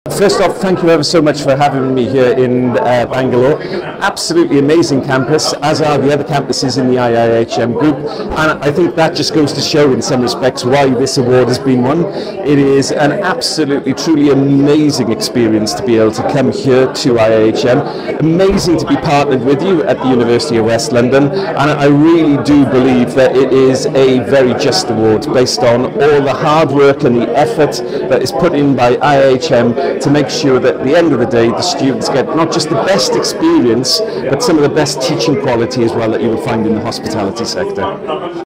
James Edmunds from University of West London visits IIHM Bangalore Campus
James Edmunds, the Associate Pro Vice-Chancellor and Dean of London Geller College of Hospitality and Tourism visited IIHM Bangalore campus and spoke about IIHM as an established entity in the field of Hospitality Education and its partnership with University of West London (UWL).

Mr. James also shares his experience on being associated with IIHM as a global hospitality education brand for years, and the partnership with UWL and how this partnership is focused on providing quality education to students and give them a global career in the field of hospitality!